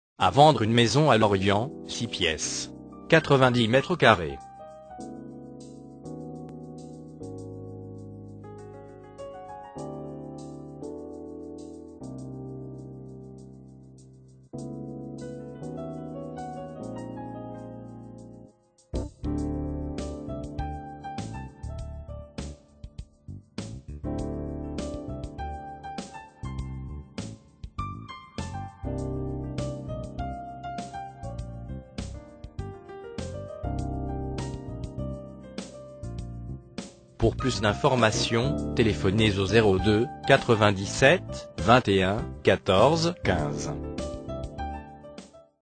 Achat Vente Maison LORIENT 56100 - 90 m2
A vendre à LORIENT une maison de 90 m2.

Annonce immobilière : Achat Vente Maison LORIENT 56100 Morbihan